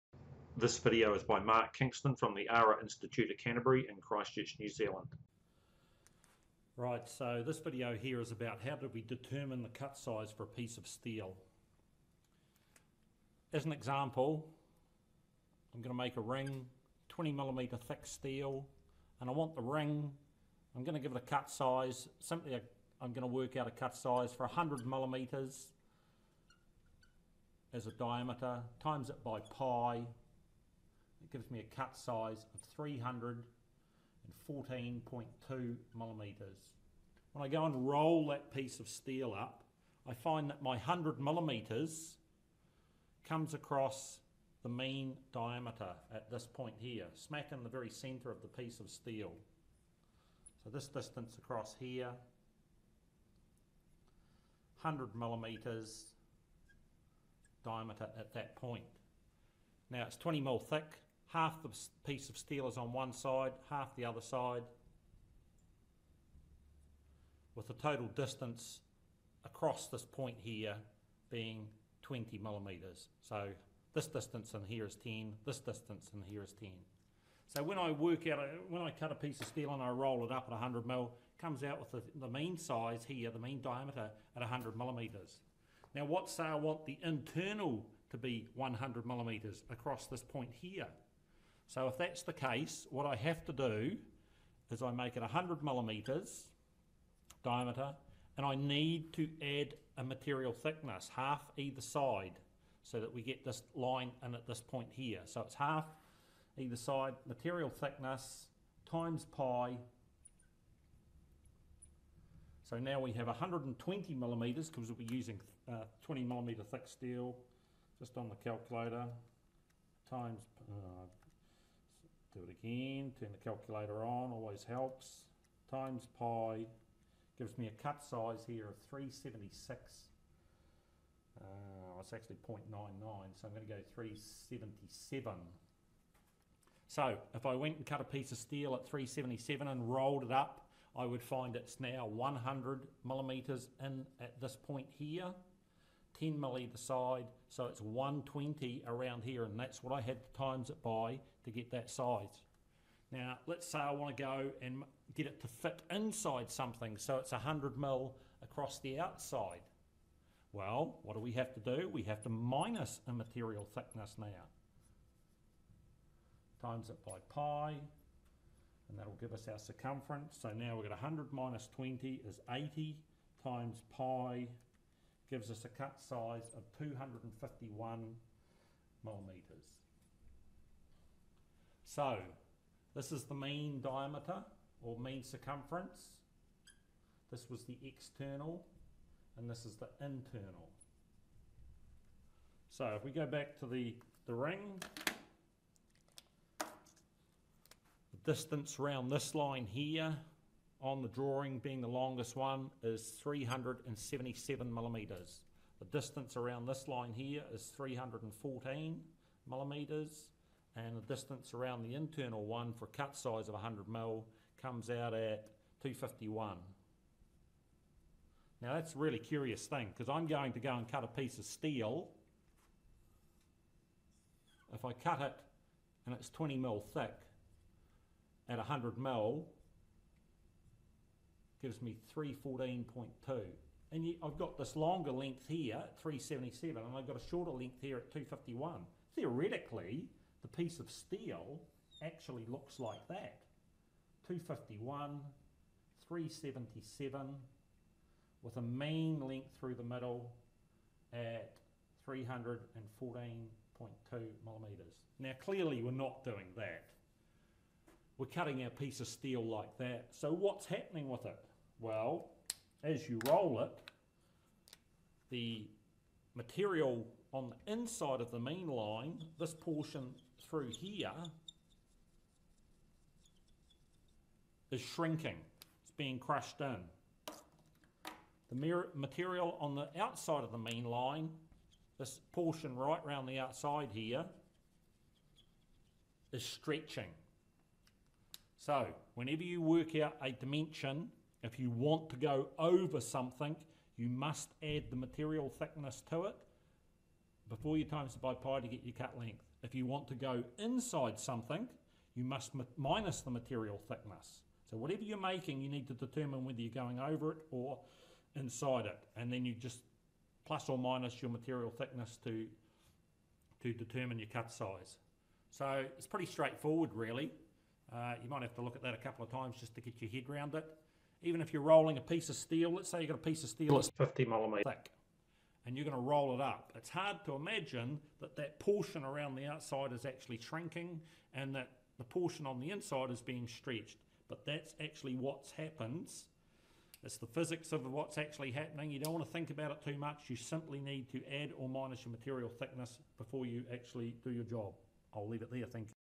How to calculate the cut length to make a rolled metal ring
A Lightboard demonstration video on how to determine the circumference cut length for a piece of metal to make a rolled ring with a given diameter.